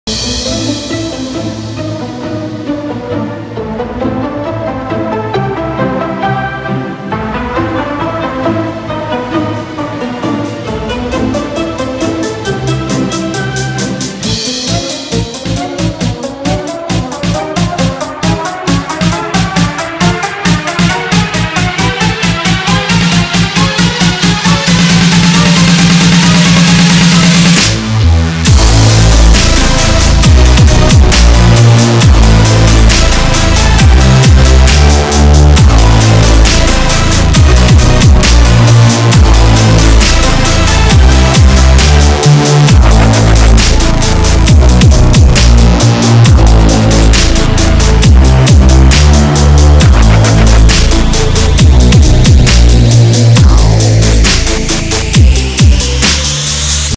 Cute funny Hedgehog | Cмешной хищный Ёжик
This cutie hedgehog was saved, cured, fed and released to the forest.